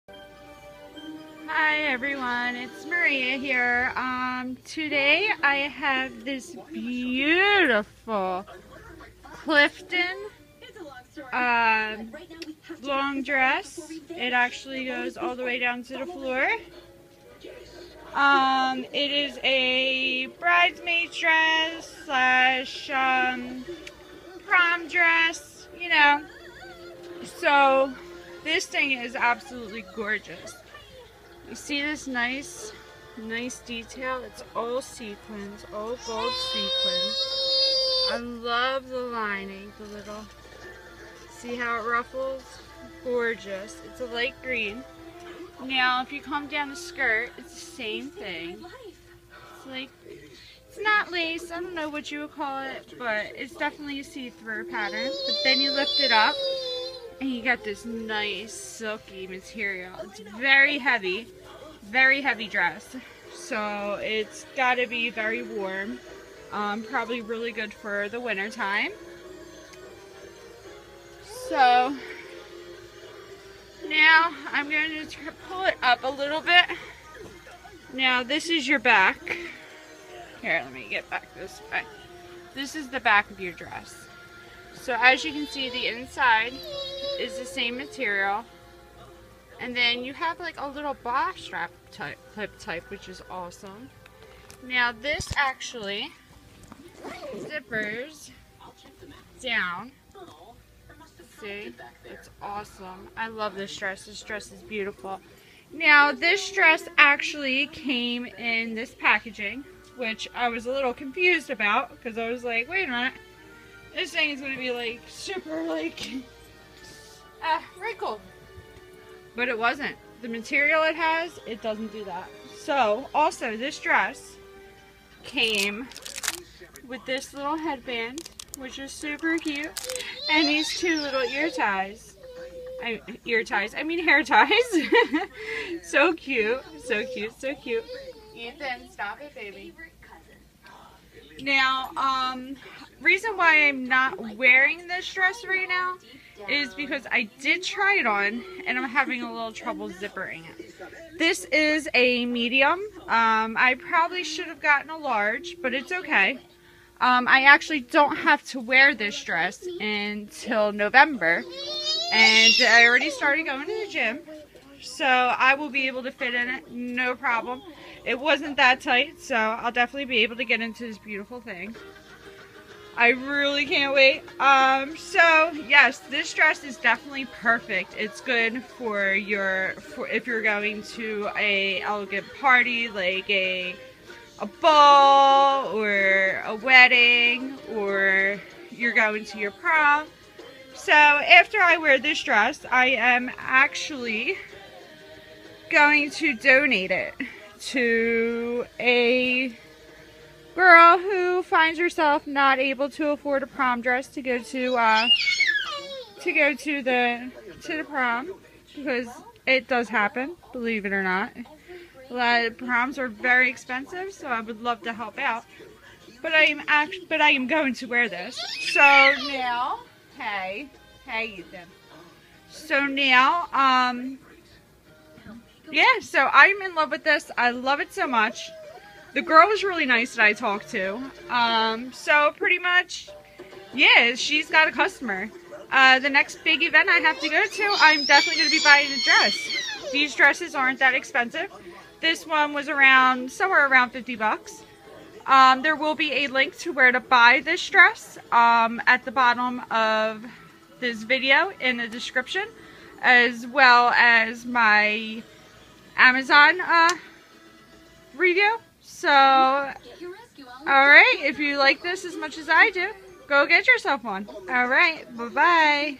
Ubridal Clifton Bridmaid Dress
Hi again!!!...It Maria!!!...So I have received this Ubridal Green Strapless Long Clifton Floor Length Brides Made Dress with Gold Beading yesterday!!!...I got this dress at a discounted price for my honest review...I picked to review this dress not only because of how beautiful it is but also because I have a wedding to attend in November...Now this dress is beautiful!!!...Very elegant!!!...At the top of this dress it has a ruffled martial with gold sequins going in a beautiful pattern all around...The skirt is two layers the first layer is really thin and flows it is also a see through material...Now the second layer is a thicker martial almost silky but a lot thicker...This dress is very heavy so good for the winter...Also it zips up the back and has a little bra strap thing inside as well...This dress is def good for fancy parties and proms!!!...Actually after I wear this dress I plan to donate it to a girl who can't afford her prom...Yea it does happen and there are real donations for that!!!...So look into it!!!...Now this dress also came with two hair ties and a headband!!!...This company has their self a new costumer!!!...These dresses are beautiful and not too expensive I believe this one was somewhere around $50...Next time I have a party I will be ordering from them!!!...

Link to Buy Product :